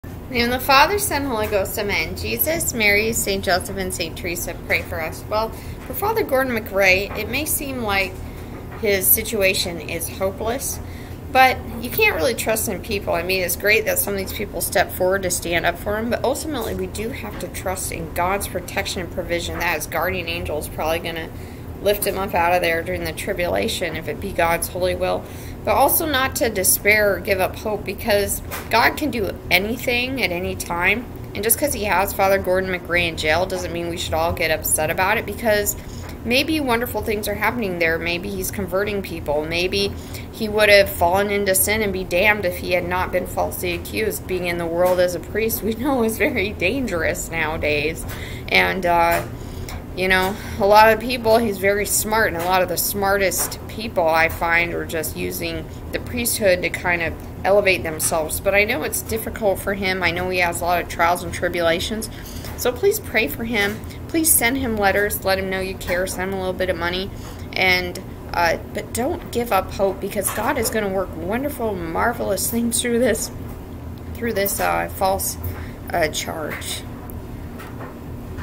God is working wonderful things through the prison ministry of Father Gordon MacRae.
Anna's twitter account (frozen): @StPhilomena7
Good morning/afternoon/evening!